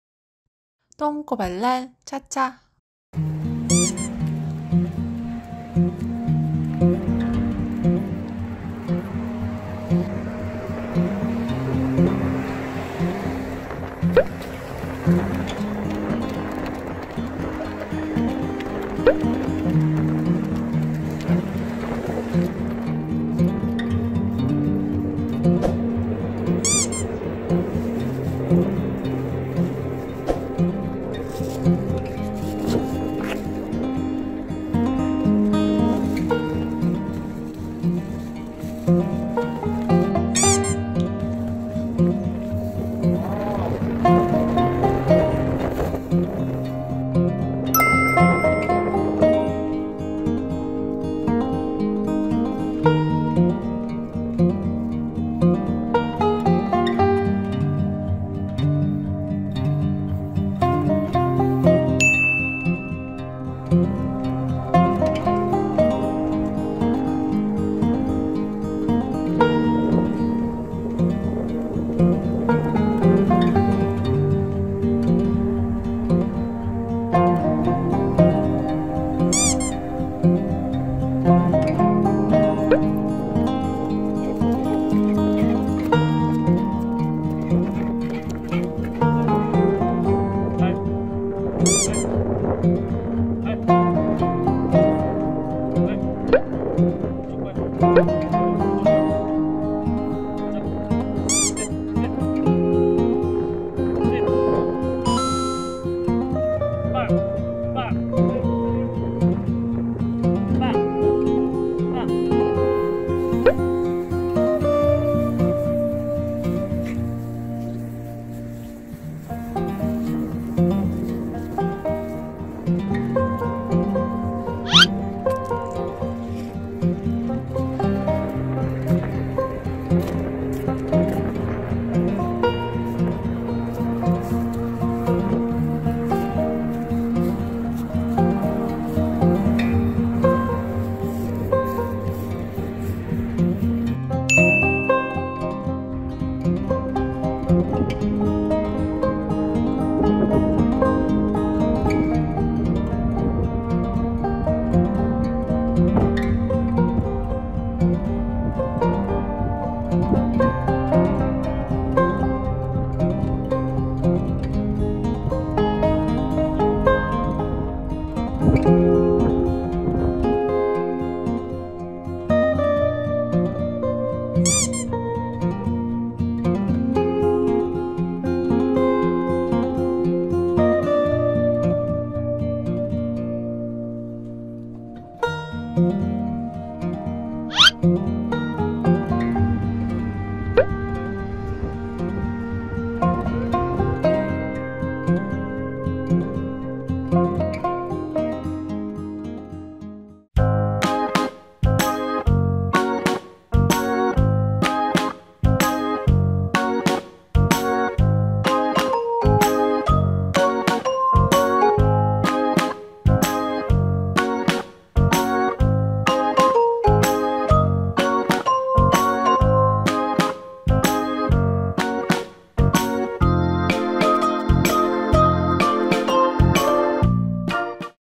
서울 전경에 너무 행복해하는 강아지 ( 한강대교 그 섬, 노들섬 산책 )
차차와 함께 노들섬 산책을 다녀왔어요~
함께 보시고 오늘도 차차와 함께 힐링하세요.

• 서울 여행지 정보
서울특별시 용산구 양녕로 445 노들섬 " 한강대교 그 섬, 노들섬 "

Golden Retriever, whose hobby is to show a deadly backside.

• Main character
name : chacha
gender : female
breed : Golden Retriever
birth : Nov. 17 2019
character : lovely, bubbly, comedy